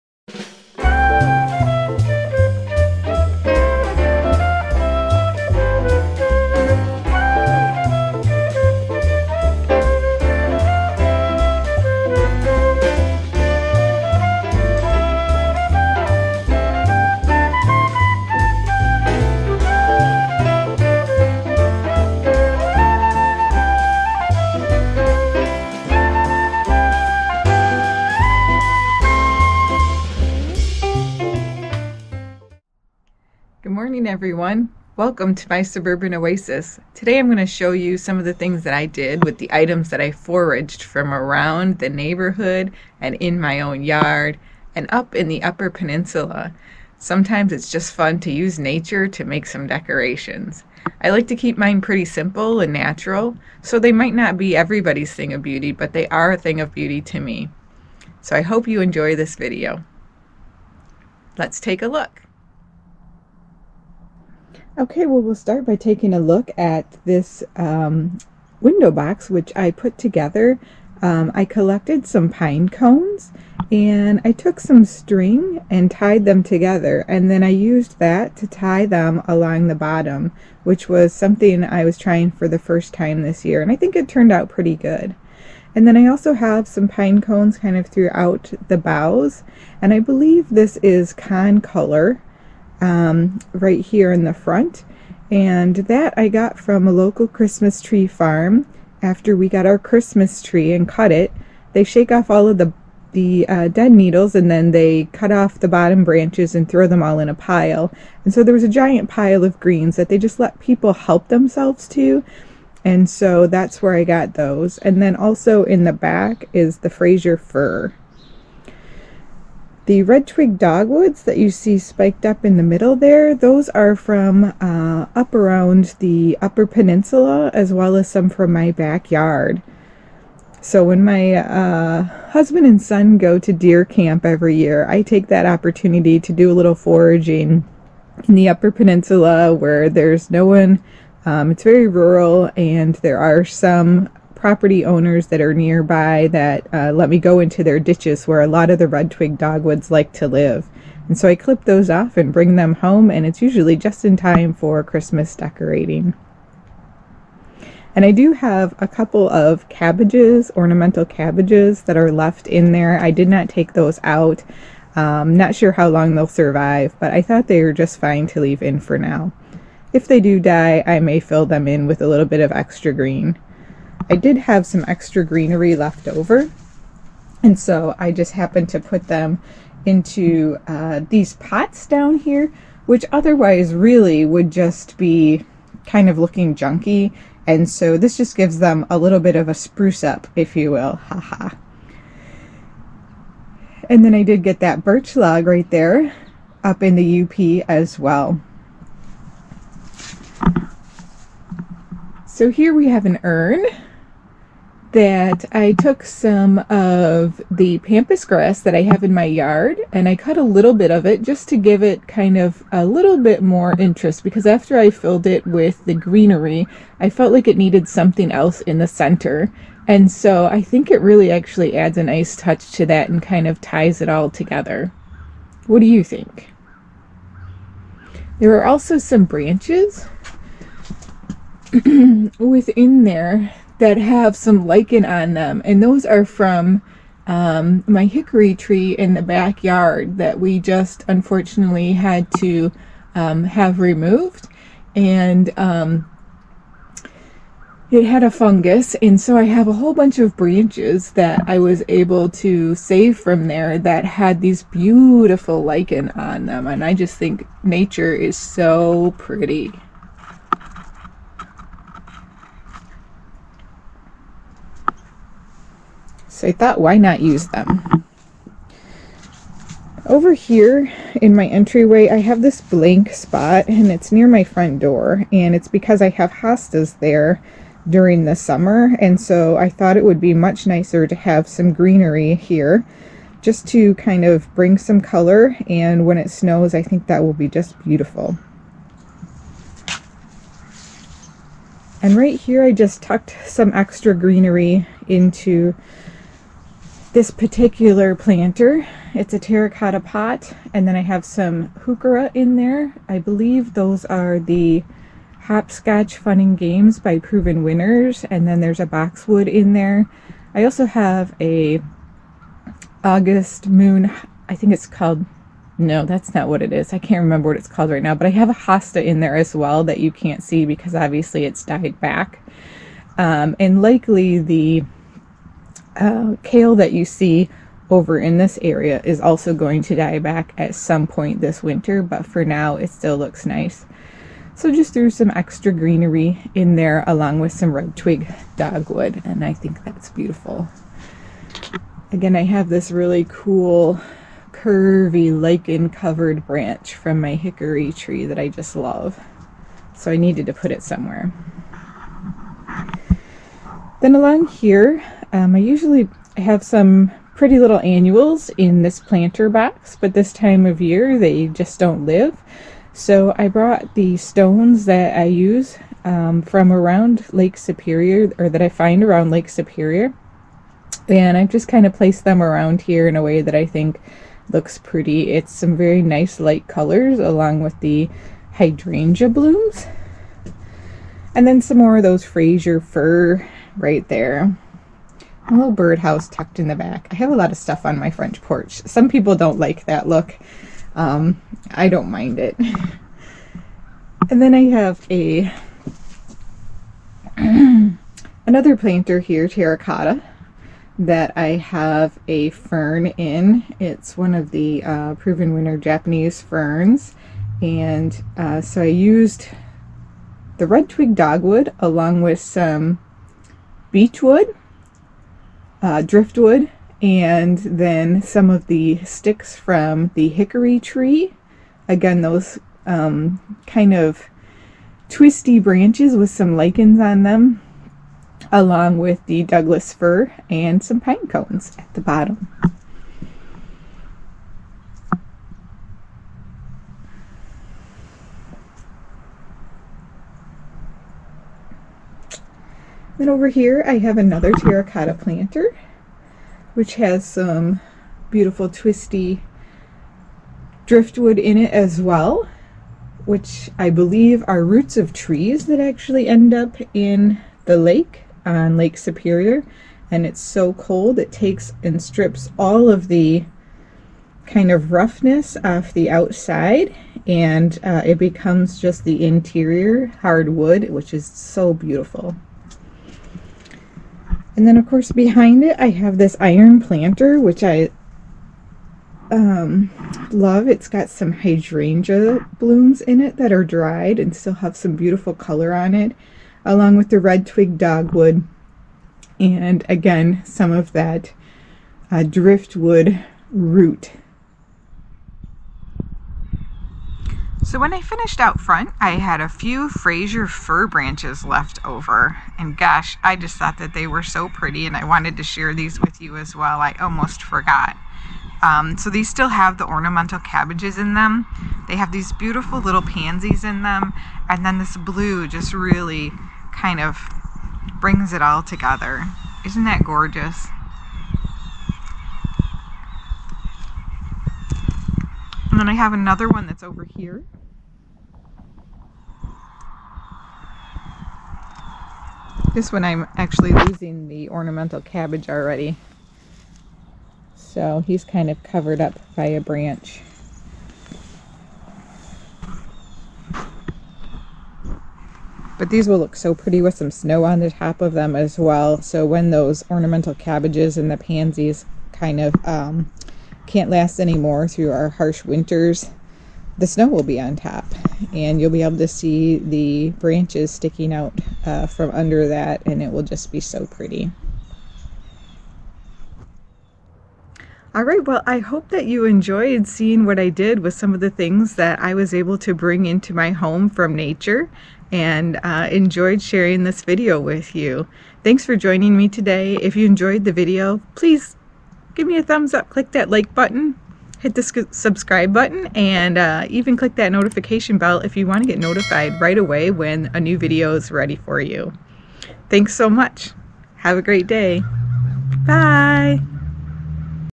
Outdoor Holiday Decorating 🏡⛄🎄 // Suburban Oasis December 2020
I had fun decorating outside for the holidays and want to share the results with you. I hope you enjoy!